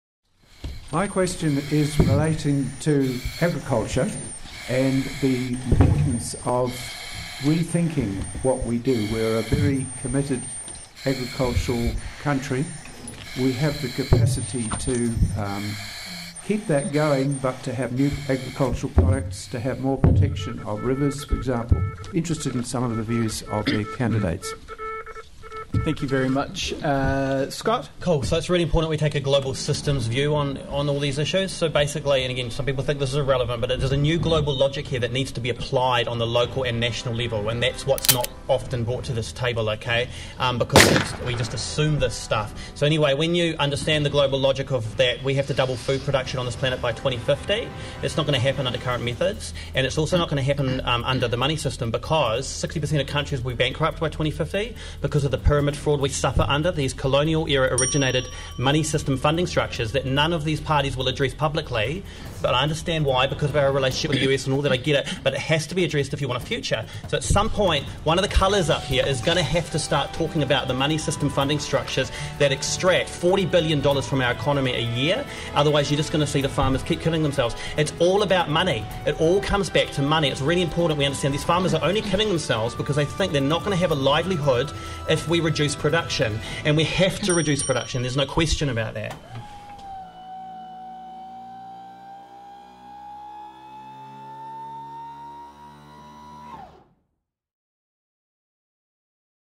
Scott Andrew on "The Suicide Machine", September 2017
From Palmerston North 'Climate Change' Candidate debate, 2017 General Election (broadcast 20/09/2017).

For further information about the *actual* solution to the growing social pathology of the market model, please visit: